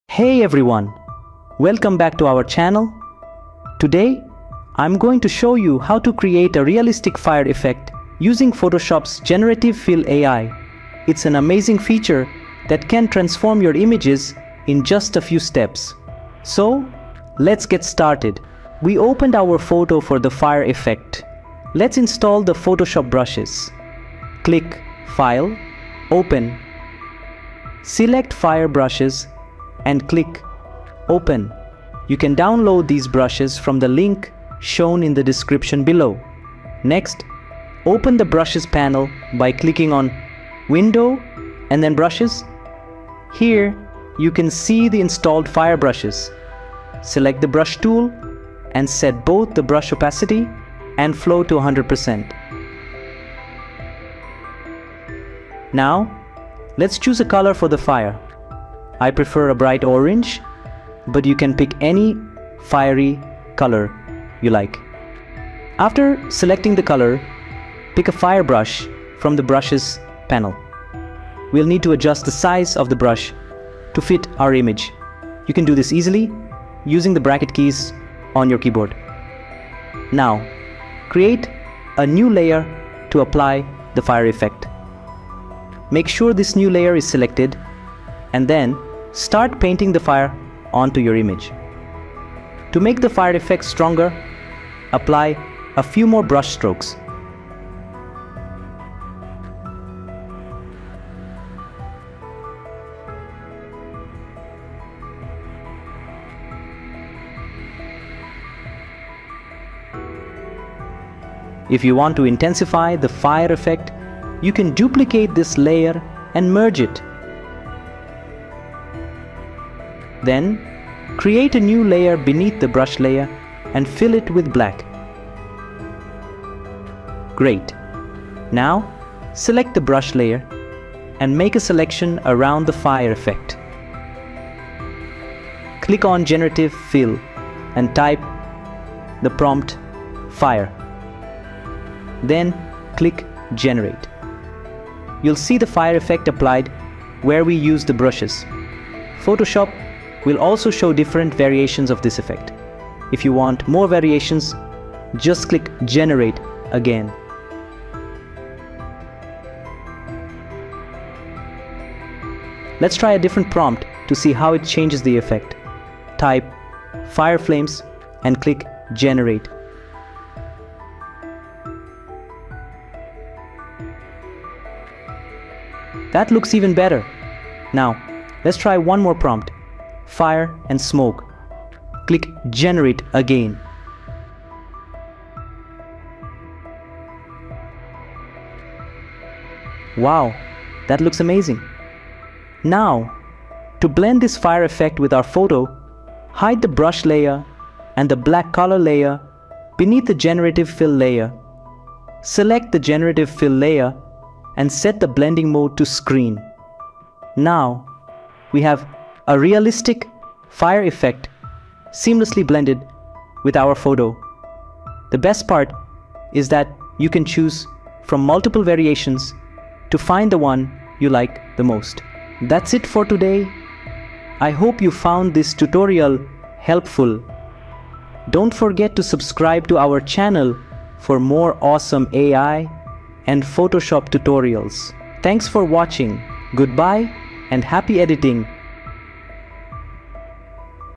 Generative Fill AI: How to Create Stunning Realistic Fire in Photoshop | Easy Photo editing tricks
In this video, we'll dive deep into the exciting world of digital art, showing you how to create realistic fire effects in Photoshop using Generative Fill AI, Photoshop brushes, and the power of Photoshop AI. Whether you're a beginner or an experienced artist, this step-by-step guide will help you master the techniques needed to bring your artwork to life with stunning fire effects.

What You'll Learn:

Introduction to Generative Fill AI -
Discover how to use this powerful tool to generate realistic fire textures and patterns.

Using Photoshop Brushes -
Learn about the best brushes for creating fire effects and how to use them effectively.

Blending and Layering Techniques -
Understand the importance of blending modes and layering to achieve a natural look.

Get insider tips on making your fire effects stand out and look more dynamic.